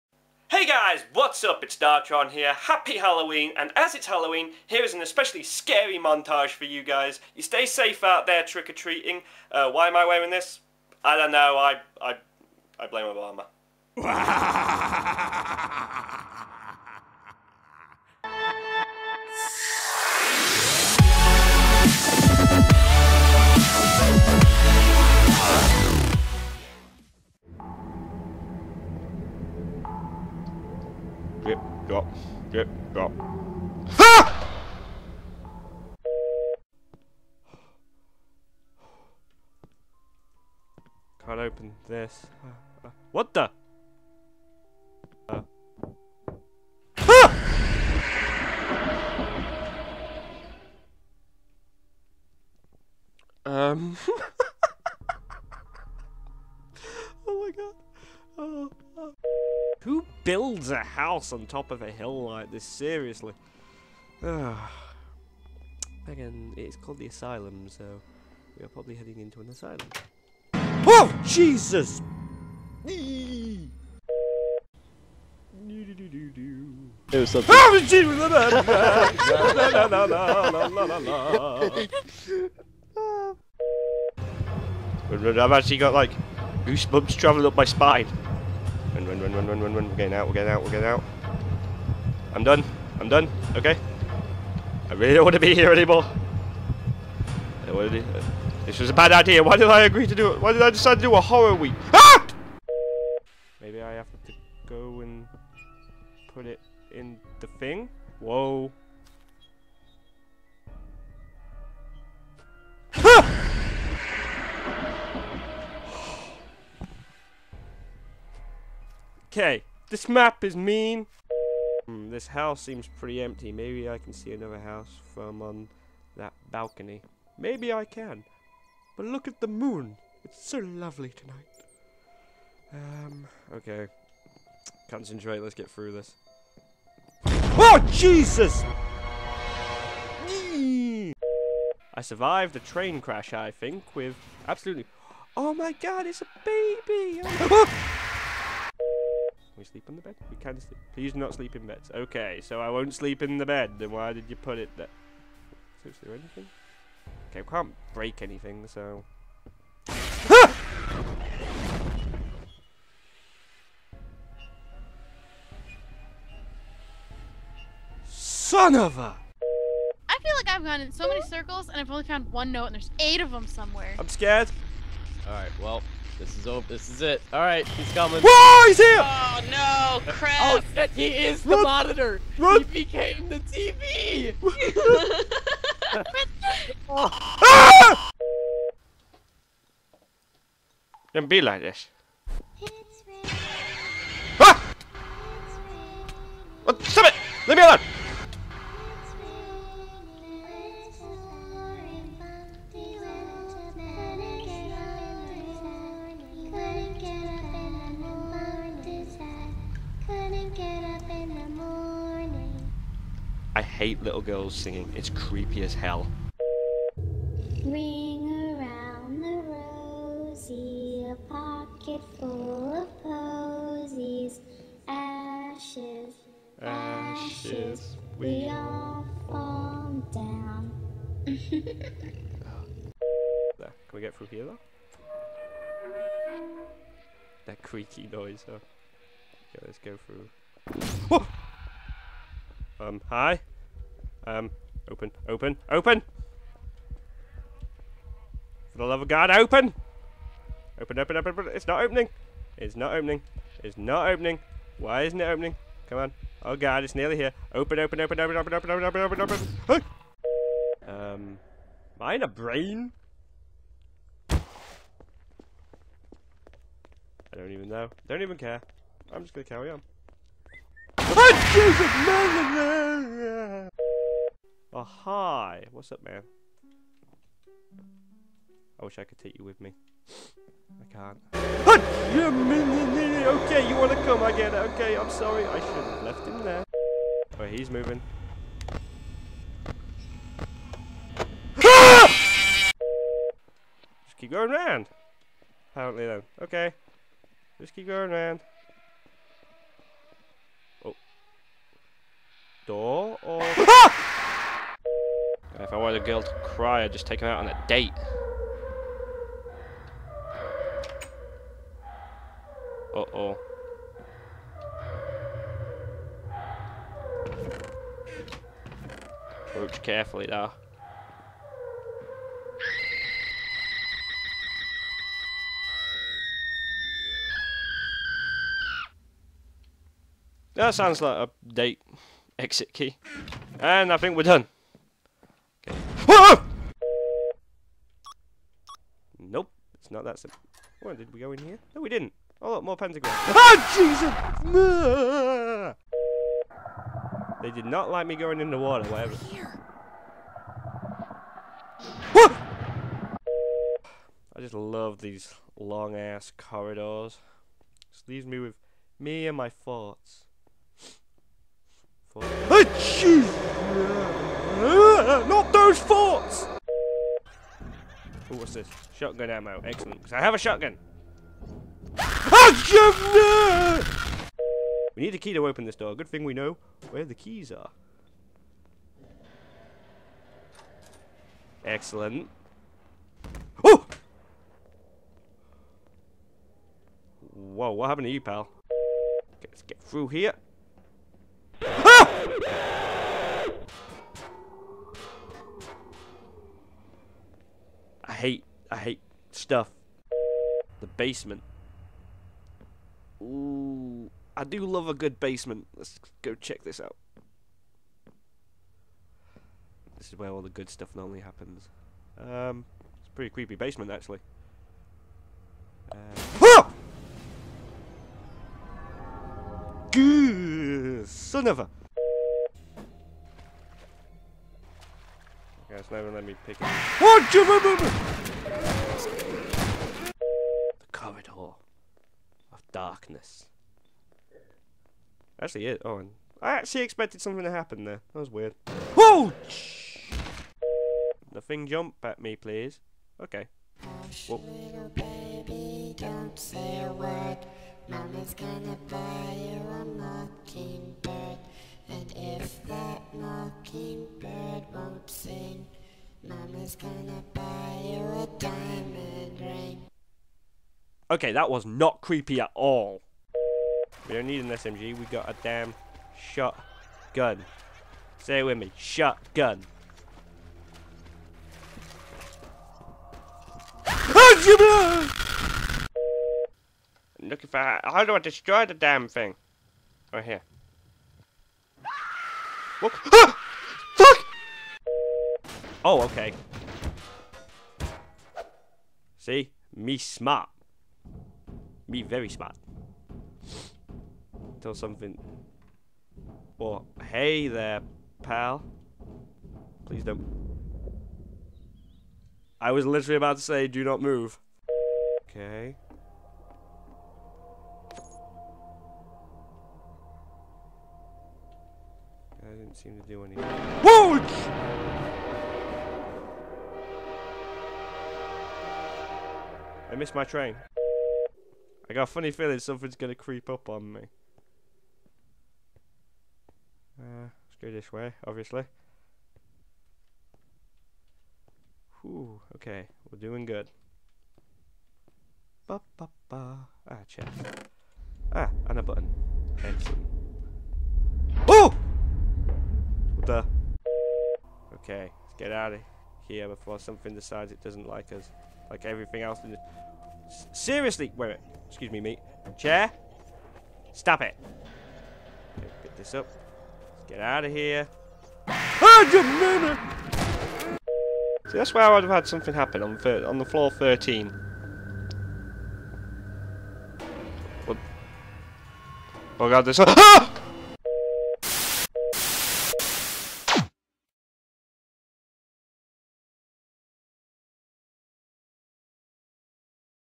HALLOWEEN HORROR MONTAGE!
JOIN ME AS WE REVISIT SOME OF THE SCARIEST MOMENTS ON THE CHANNEL. GHOULS, GHOSTS AND MONSTERS AWAIT US!